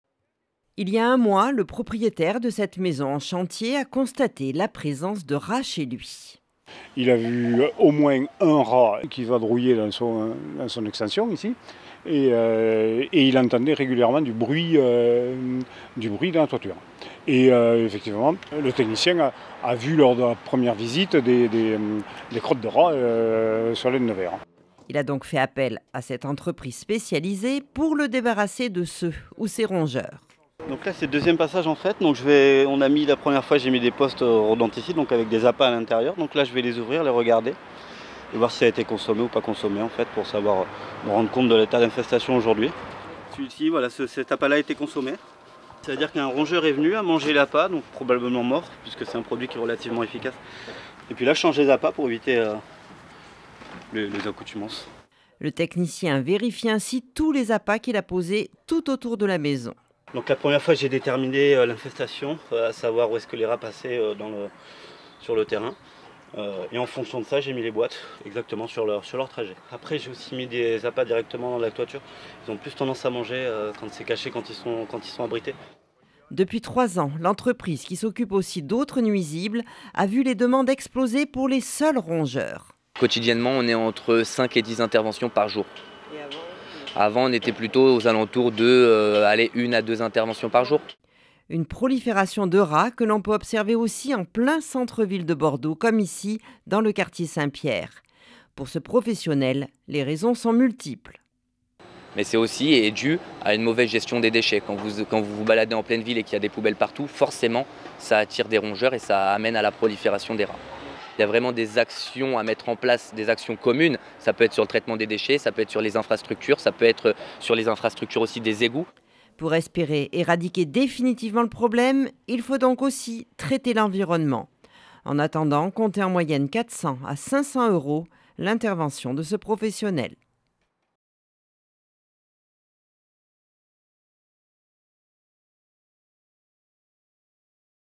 Rats dans la métropole bordelaise : opération dératisation
Depuis environ 3 ans, les rats sont de plus en plus visibles dans nos villes et notamment dans la métropole bordelaise. On parle de 500 000 rongeurs qui prolifèrent dans l’agglomération. Pour les professionnels, les demandes d’intervention des particuliers pour les débarrasser de ces nuisibles ont explosé. Un problème pas facile à éra...diquer